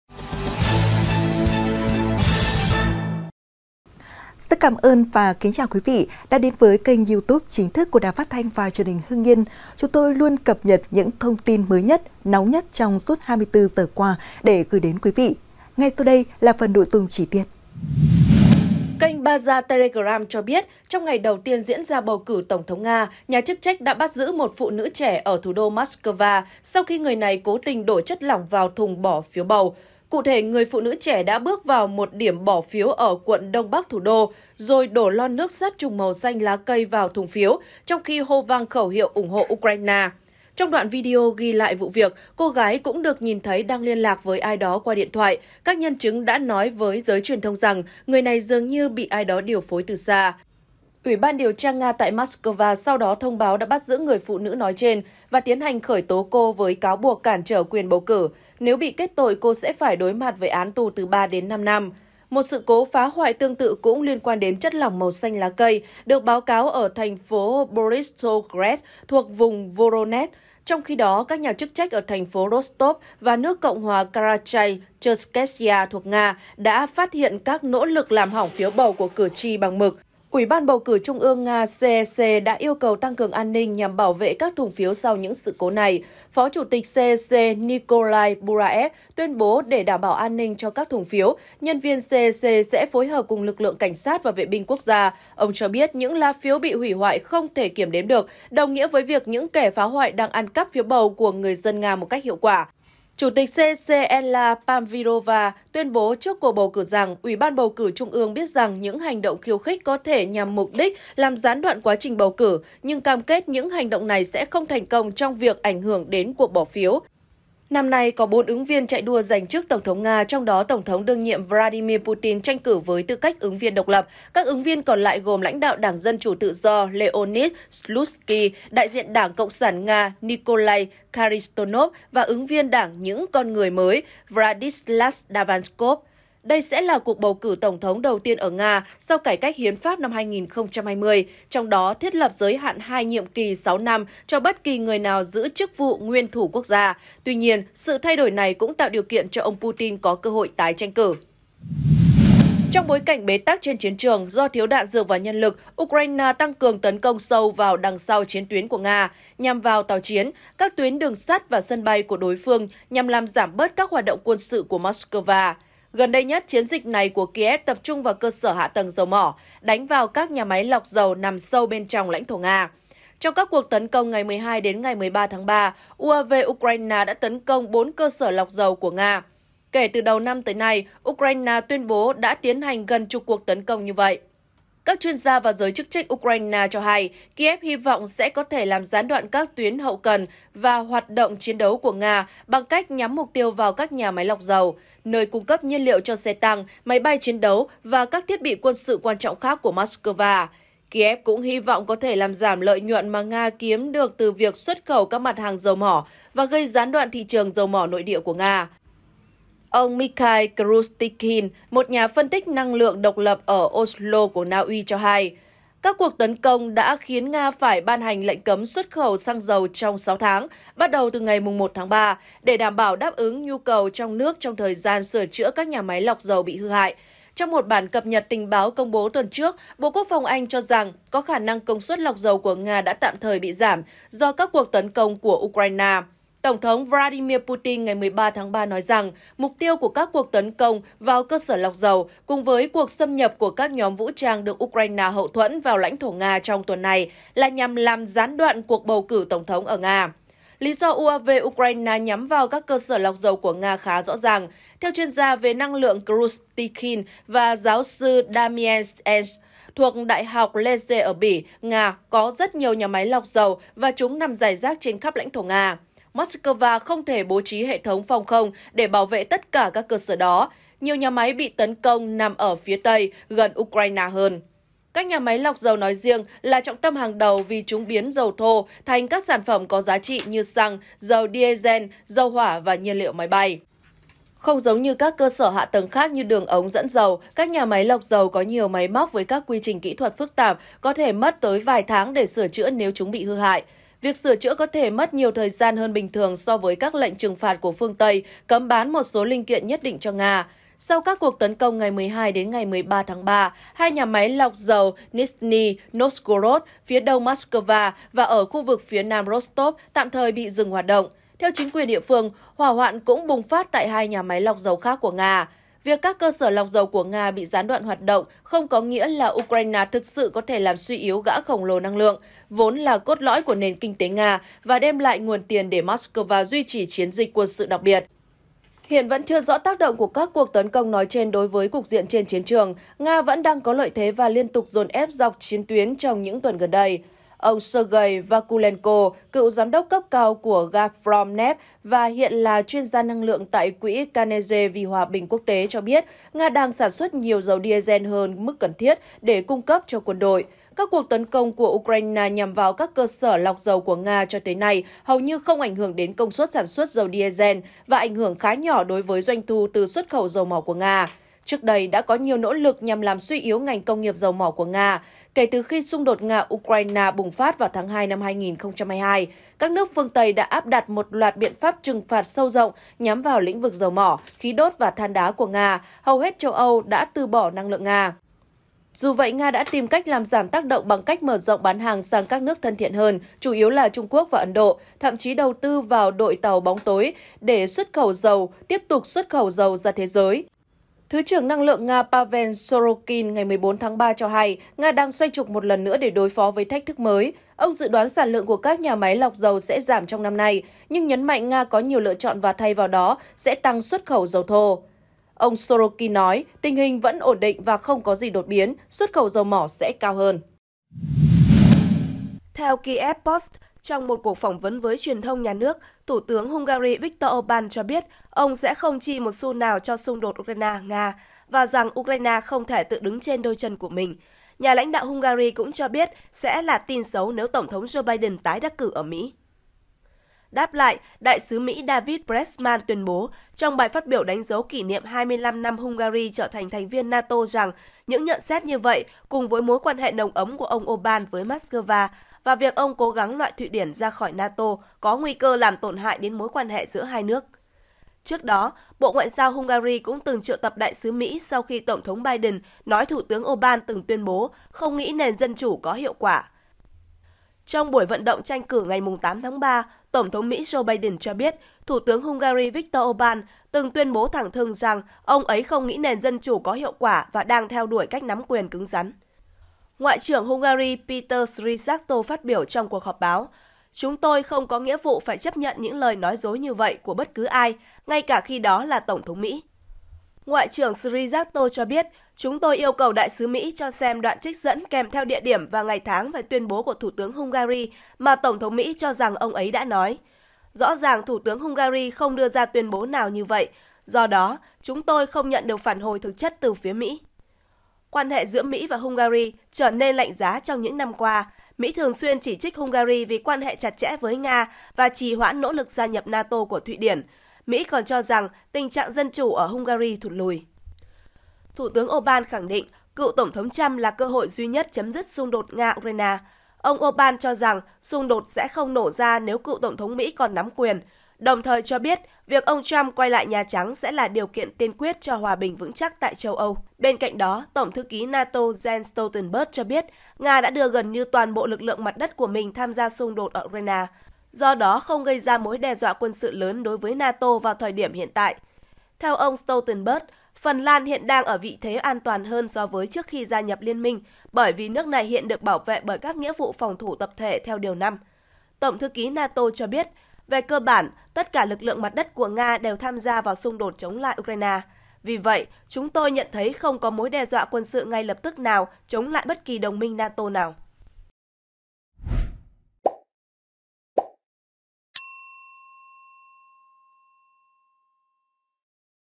Tin quốc tế: Nga phát hiện âm mưu đổ ‘chất lạ’ phá bầu cử, liên tiếp các "con rối" bị bắt
Bầu cử Nga mới nhất | Tin quốc tế: Nga phát hiện âm mưu đổ ‘chất lạ’ phá bầu cử, liên tiếp các "con rối" bị bắt
Kênh Baza Telegram cho biết, trong ngày đầu tiên diễn ra bầu cử tổng thống Nga, nhà chức trách đã bắt giữ một phụ nữ trẻ ở thủ đô Moscow sau khi người này cố tình đổ chất lỏng vào thùng bỏ phiếu bầu.
Địa chỉ: 164 Nguyễn Văn Linh, thành phố Hưng Yên, tỉnh Hưng Yên
Tiktok: Truyền hình Hưng Yên
Facebook: Đài Phát thanh và Truyền hình Hưng Yên
Zalo: Đài PTTH Hưng Yên